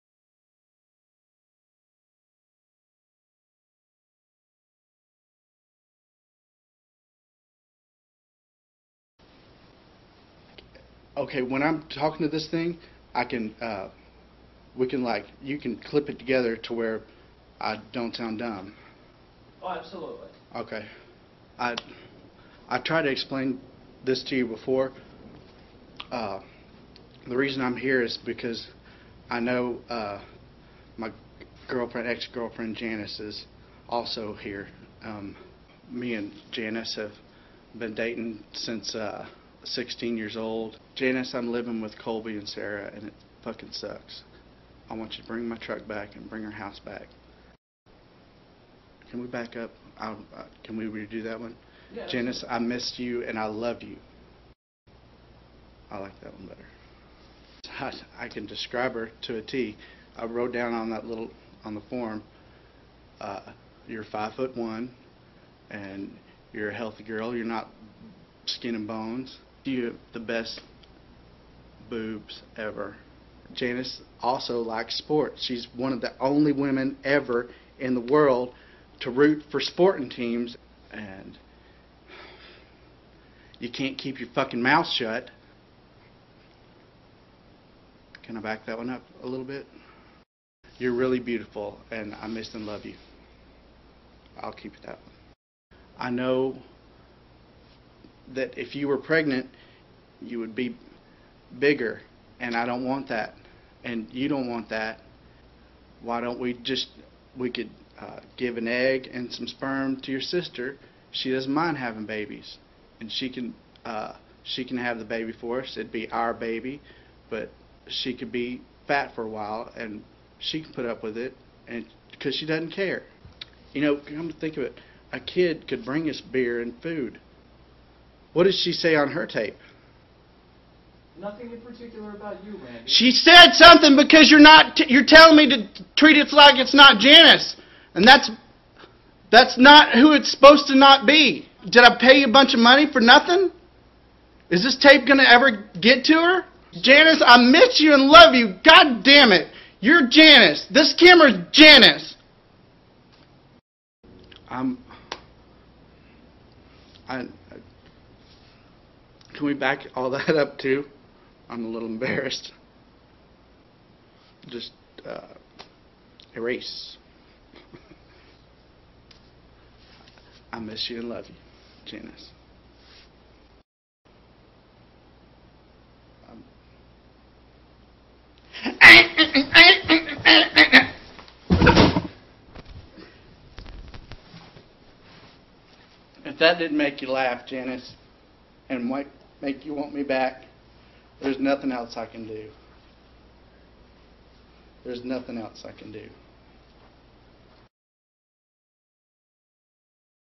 Funny Redneck Video Dating Disaster
After being dumped this is not a good way to convince your ex to give you another chance.